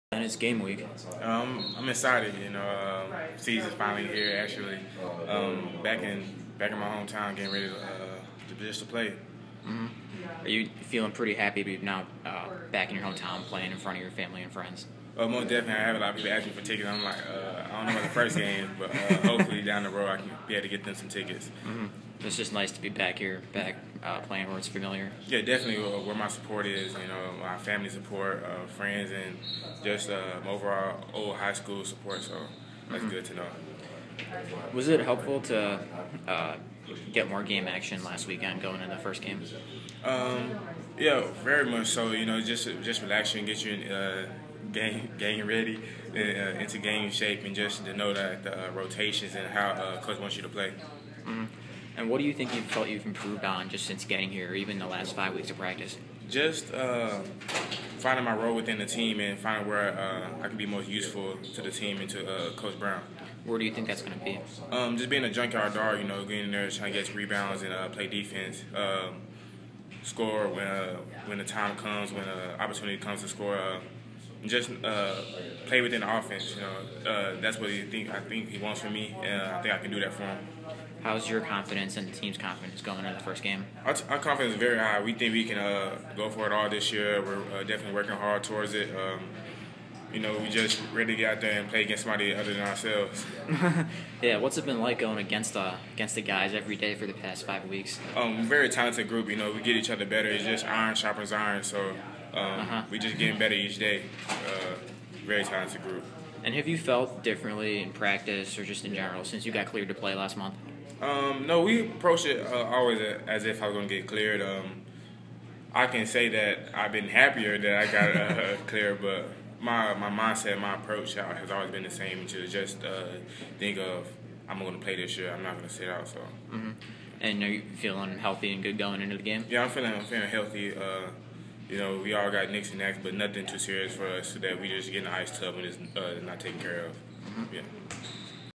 Ben Emelogu Q&A
SMU G Ben Emelogu speaks before practice on Nov. 11, 2014.